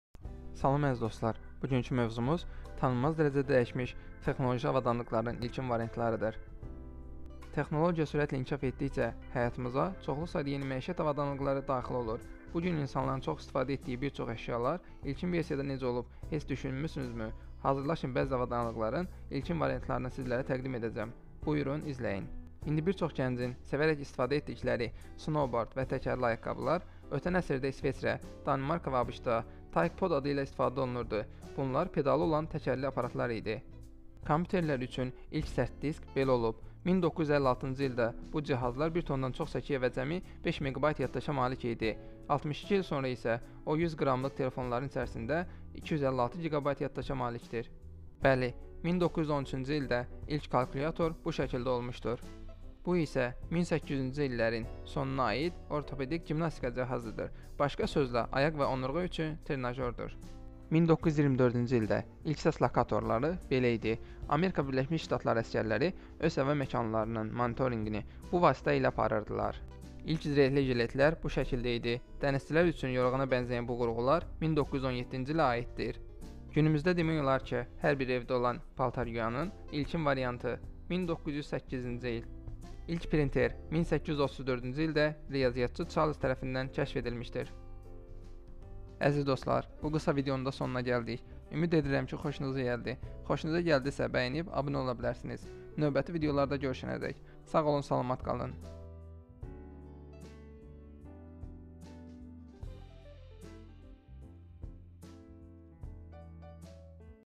Tanınmaz dərəcədə dəyişmiş texnoloji avadanlıqların ilkin variantları
Dostlar Salam! ツ

Texnologiyalar sürətlə inkişaf etdikcə həyatımıza çoxlu sayda yeni məişət avadanlıqları daxil olur. Bu gün insanların  çox istifadə etdiyi bir çox əşyalar ilkin versiyada necə olub, heç düşünmüsünüzmü? Hazırlaşın bəzi avadanlıqların ilkin variantın sizlərə təqdim edəcəm. Buyurun izləyin.

Ümidvaram ki xoşunuza gələcək. Xoşunuza gəldisə "bəyən"məyi, "şərh" yazmağı və "abonə" olmağı unutmayın.

Gələcəkdə görmək istədiyiniz birşeyləri şərhdə qeyd etməyi unutmayın (◑‿◐) .